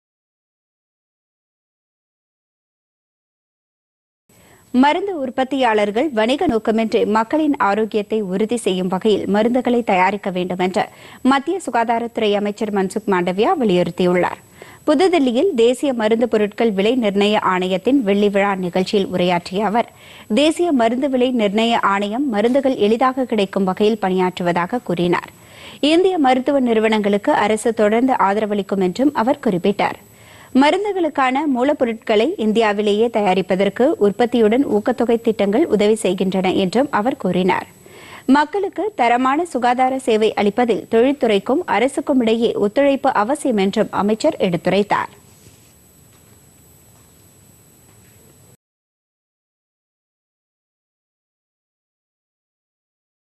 வணிக நோக்கமின்றி மருந்து உற்பத்திPodhigaiTamilNews
மருந்து உற்பத்தியாளர்கள் வணிக நோக்கமின்றி மக்களின் ஆரோக்கியத்ததை உறுதி செய்யும் வகையில் மருந்துகளை தயாரிக்க வேண்டும் -மத்திய சுகாதாரத்துறை அமைச்சர் மன்சுக் மாண்டவியா வலியுறுத்தல்.